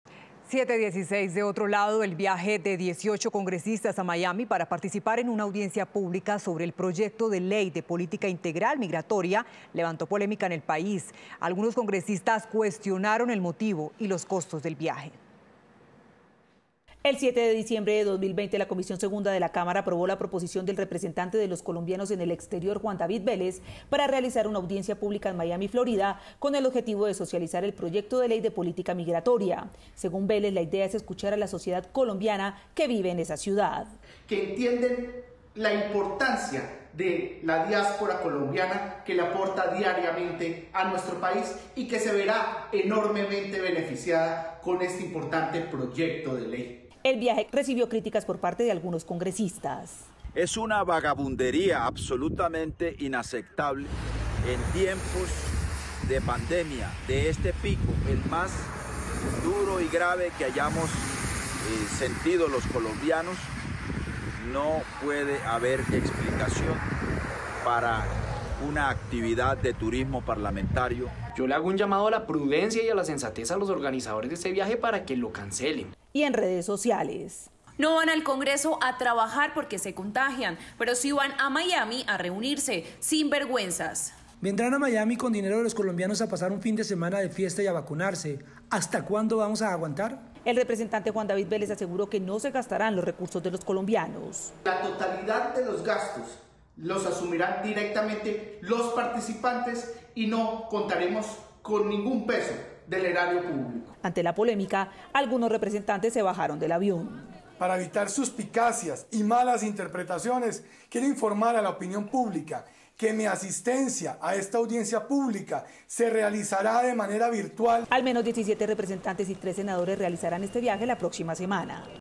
Controversia por viaje de congresistas a Miami con el fin de "socializar proyecto de ley migratoria"
El viaje de 18 congresistas a Miami para participar en una audiencia pública sobre el proyecto de ley de política integral migratoria levantó polémica en el país. Algunos congresistas cuestionaron el motivo y los costos del viaje.